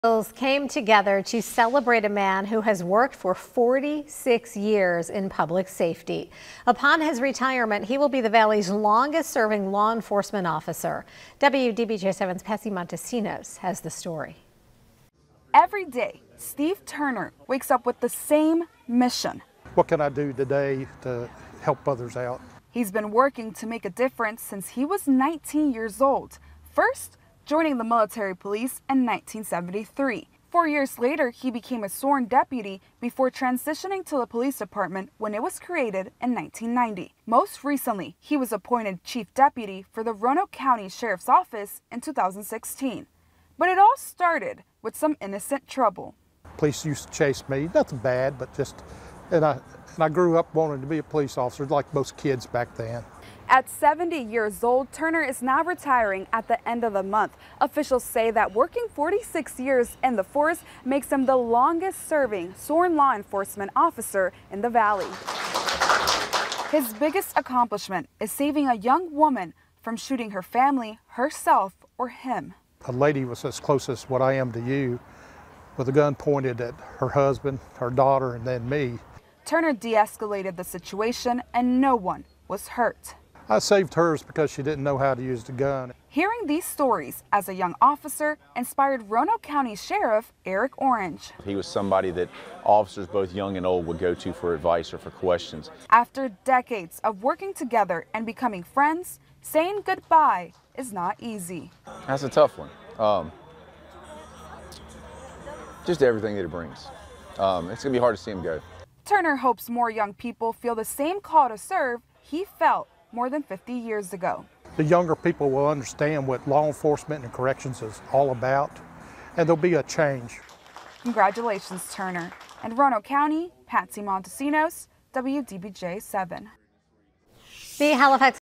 Roanoke County’s longest-serving law enforcement officer retires after 46 years
ROANOKE, Va. (WDBJ) - Roanoke County officials came together Friday to celebrate a man who has worked for 46 years in public safety.